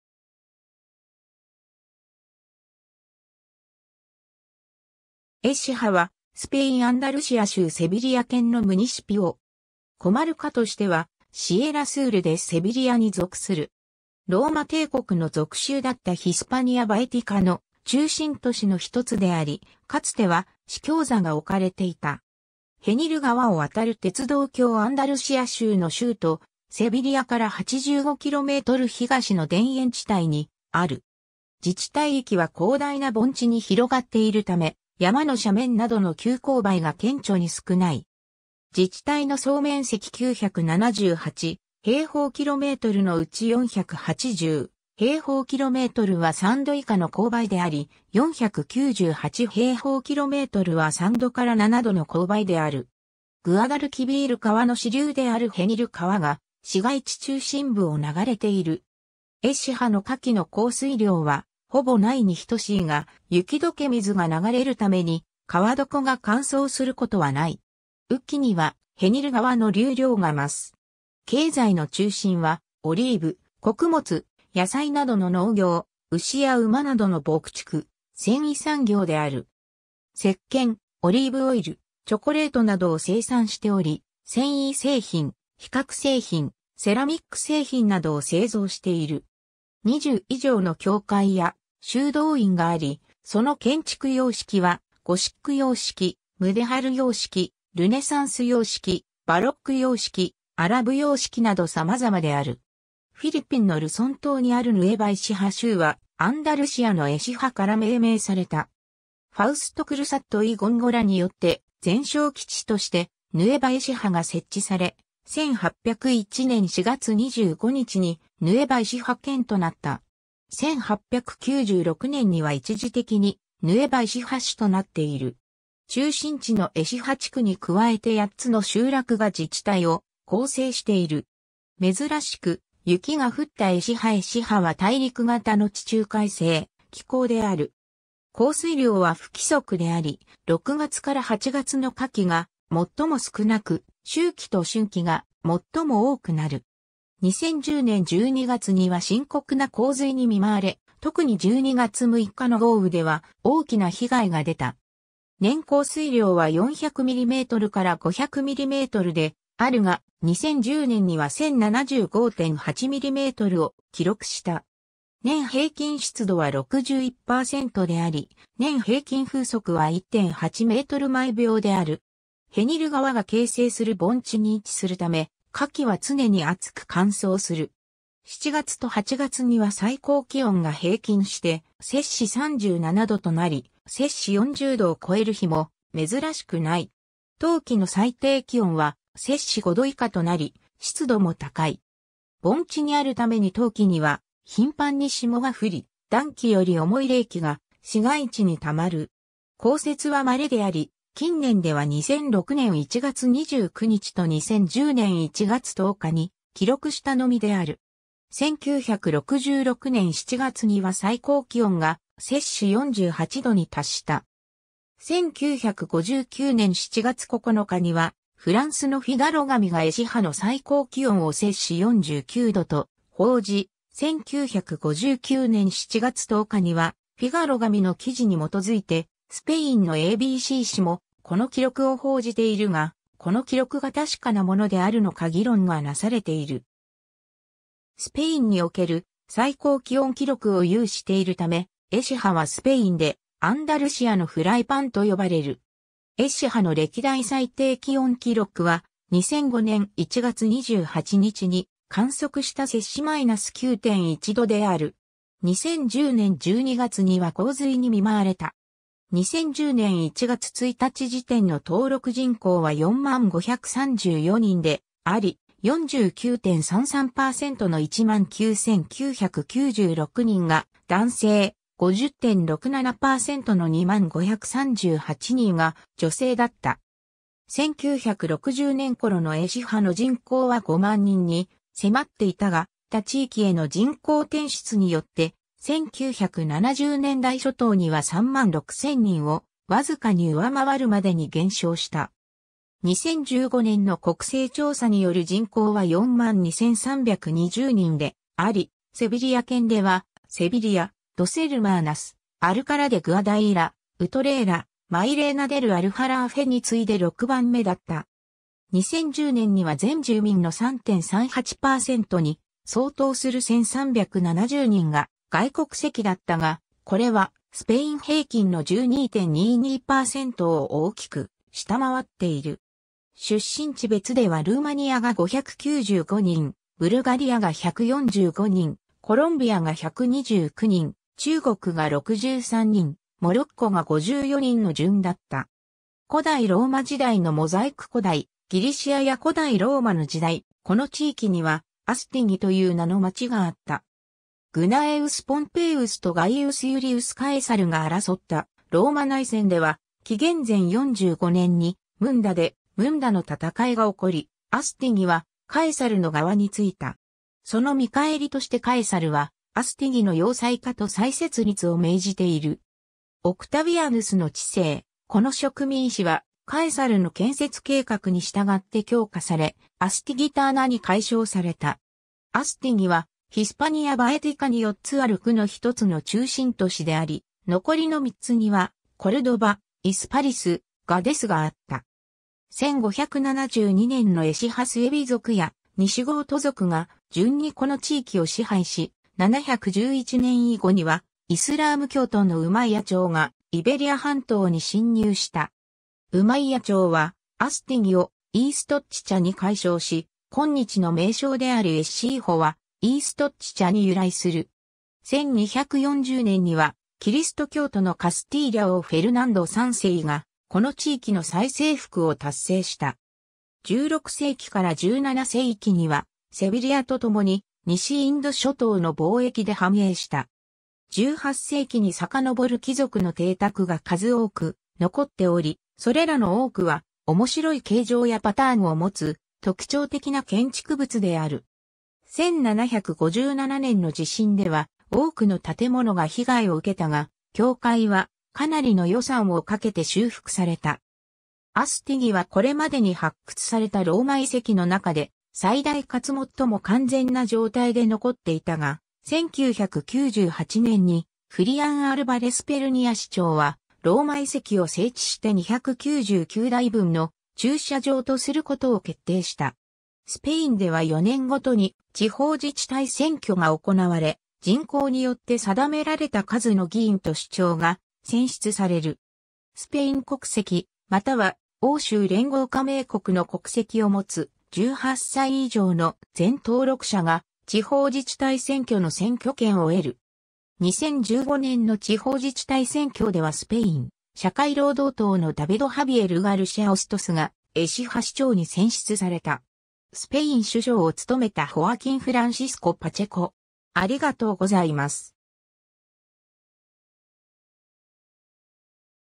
エシハ
エシハ（スペイン語: Écija）は、スペイン・アンダルシア州セビリア県のムニシピオ（基礎自治体）。
コマルカ（郡）としてはシエラ・スール・デ・セビリアに属する。
ローマ帝国の属州だったヒスパニア・バエティカの中心都市のひとつであり、かつては司教座がおかれていた。
 ヘニル川を渡る鉄道橋 アンダルシア州の州都セビリアから85km東の田園地帯にある。
自治体域は広大な盆地に広がっているため、山の斜面などの急勾配が顕著に少ない。
自治体の総面積978km2のうち480km2は3度以下の勾配であり、498km2は3度-7度の勾配である。
グアダルキビール川の支流であるヘニル川が市街地中心部を流れている。
エシハの夏季の降水量はほぼないに等しいが、雪解け水が流れるために河床が乾燥することはない。
雨季にはヘニル川の流量が増す。
 経済の中心は、オリーブ・穀物・野菜などの農業、牛や馬などの牧畜、繊維産業である。
石鹸、オリーブオイル、チョコレートなどを生産しており、繊維製品、皮革製品、セラミック製品などを製造している。
 20以上の教会や修道院があり、その建築様式はゴシック様式、ムデハル様式、ルネサンス様式、バロック様式、アラブ様式など様々である。
 フィリピンのルソン島にあるヌエヴァ・エシハ州はアンダルシアのエシハから命名された。
ファウスト・クルサット・イ・ゴンゴラ（1690年-1701年）によって前哨基地としてヌエヴァ・エシハが設置され、1801年4月25日にヌエヴァ・エシハ県となった。
1896年には一時的にヌエヴァ・エシハ市となっている。
 中心地のエシハ地区に加えて8つの集落が自治体を構成している。
 珍しく雪が降ったエシハ（2006年1月29日） エシハは大陸型の地中海性気候である。
降水量は不規則であり、6月から8月の夏季がもっとも少なく、秋季と春季がもっとも多くなる。
2010年12月には深刻な洪水に見舞われ、特に12月6日の豪雨では大きな被害が出た。
年降水量は400mm-500mmであるが、2010年には1,075.8mmを記録した。
年平均湿度は61%であり、年平均風速は1.8m/秒である。
 ヘニル川が形成する盆地に位置するため、夏季は常に暑く乾燥する。
7月と8月には最高気温が平均して摂氏37度となり、摂氏40度を超える日も珍しくない。
冬季の最低気温は摂氏5度以下となり、湿度も高い。
盆地にあるために冬季には頻繁に霜が降り、暖気より重い冷気が市街地にたまる。
降雪は稀であり、近年では2006年1月29日と2010年1月10日に記録したのみである。
 1966年7月には最高気温が摂氏48度に達した。
1959年7月9日にはフランスのフィガロ紙がエシハの最高気温を摂氏49度と報じ、1959年7月10日にはフィガロ紙の記事に基づいてスペインのABC紙もこの記録を報じているが、この記録が確かなものであるのか議論がなされている。
スペインにおける最高気温記録を有しているため、エシハはスペインで「アンダルシアのフライパン」と呼ばれる。
 エシハの歴代最低気温記録は、2005年1月28日に観測した摂氏マイナス9.1度である。
2010年12月には洪水に見舞われた。
 2010年1月1日時点の登録人口は40,534人であり、49.33%の19,996人が男性、50.67%の20,538人が女性だった。
1960年頃のエシハの人口は50,000人に迫っていたが、他地域への人口転出によって、1970年代初頭には36,000人をわずかに上回るまでに減少した。
2015年の国勢調査による人口は42,320人であり、セビリア県ではセビリア、ドス・エルマーナス、アルカラ・デ・グアダイーラ、ウトレーラ、マイレーナ・デル・アルハラーフェに次いで6番目だった。
 2010年には全住民の3.38%に相当する1,370人が外国籍だったが、これはスペイン平均の12.22%を大きく下回っている。
出身地別ではルーマニアが595人、ブルガリアが145人、コロンビアが129人、中国が63人、モロッコが54人の順だった。
 古代ローマ時代のモザイク 古代ギリシアや古代ローマの時代、この地域にはアスティギ(Astigi)という名の街があった。
グナエウス・ポンペイウスとガイウス・ユリウス・カエサルが争ったローマ内戦では、紀元前45年にムンダ（現・オスナ）でムンダの戦いが起こり、アスティギはカエサルの側に付いた。
その見返りとしてカエサルは...